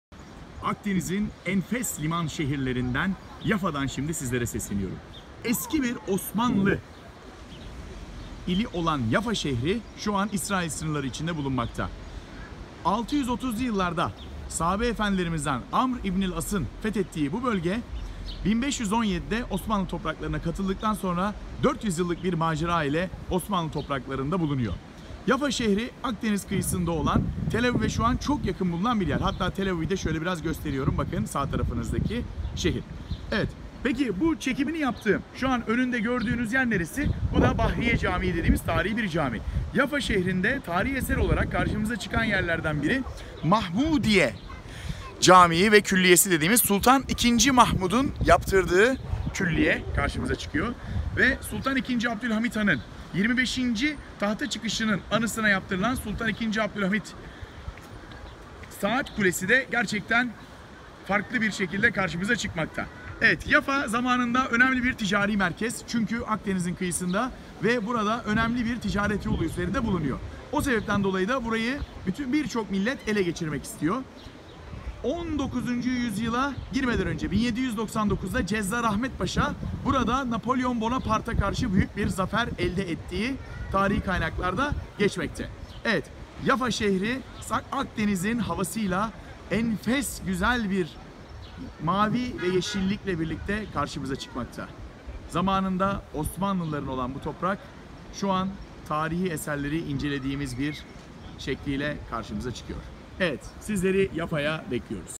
TARİHİ OSMANLI ŞEHRİ YAFA
İlk olarak 7. yüzyılda bir sahabe efendimizin fethettiği denilen ve sonrasında 1517’den itibaren 1917’lere kadar bizde kalan Güzelim Akdeniz Liman Şehri..
Adını Nuh aleyhisselam oğlu Yafes’ten aldığı söylenmekte..